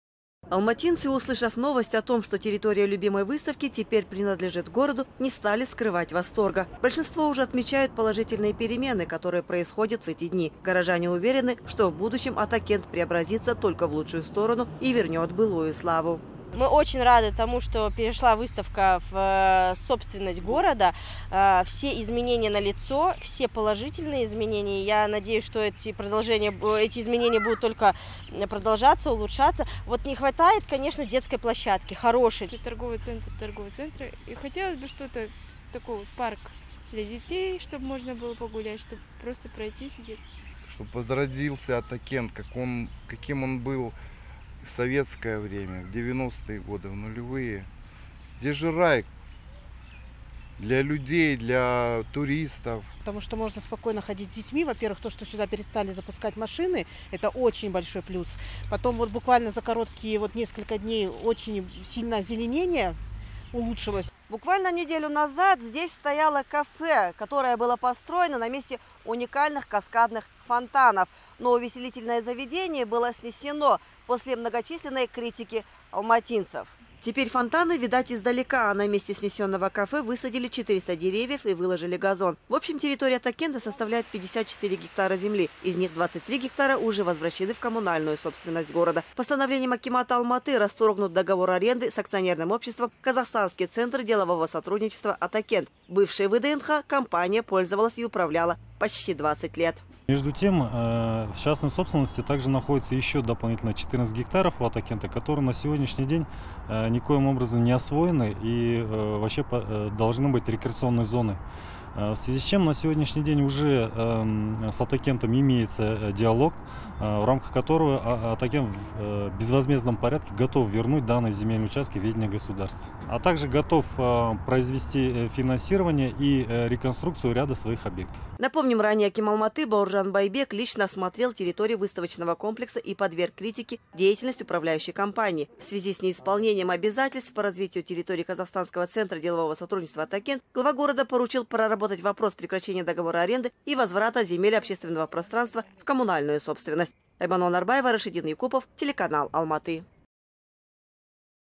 Территория Атакента перешла в коммунальную собственность города (03.05.18)
Земельный фонд Алматы увеличился на 23 гектара. Именно такой участок передан в коммунальную собственность города - это территория Атакента. Вскоре ожидается ещё 14 гектаров земли. Впрочем, положительные изменения алматинцы уже ощутили.